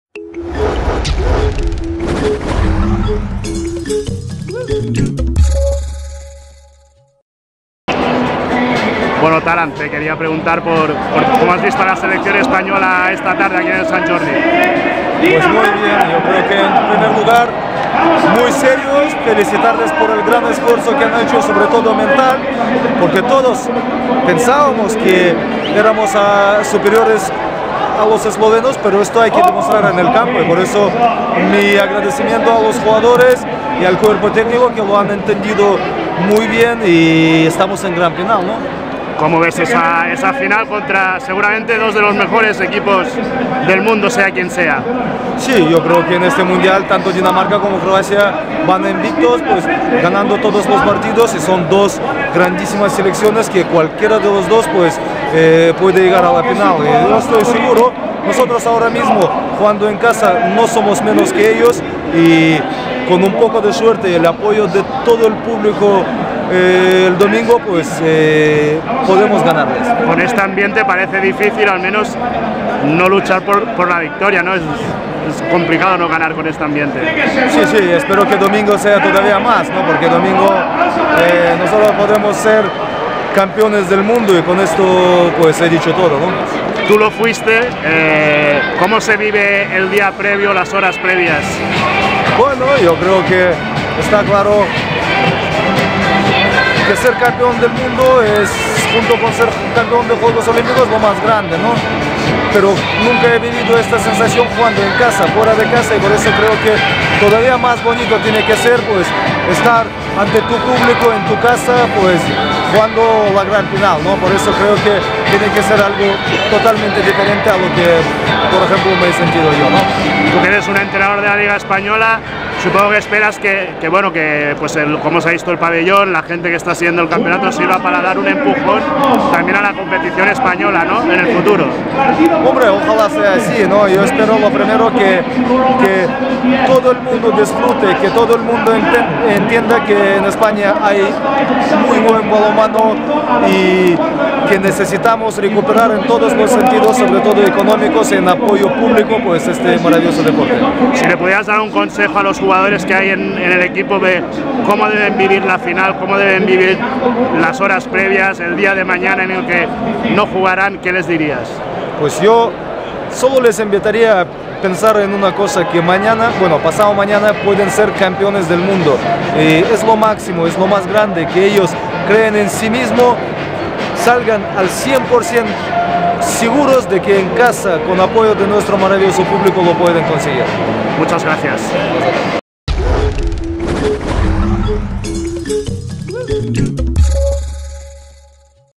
Entrevista a Talant Dujshebaev
El técnico del BM Atlético de Madrid nos da sus impresiones sobre el torneo mundialista.